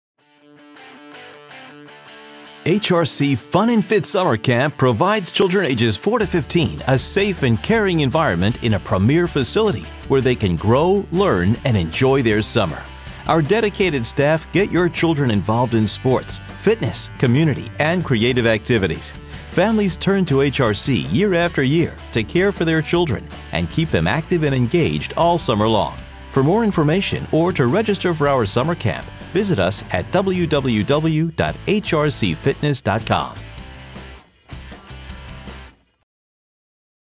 Summer Camp At HRC Fitness
HRC Fitness offers a youth summer camp filled with activities your kids will be talking about for months. We are located at 30 Brower Ln.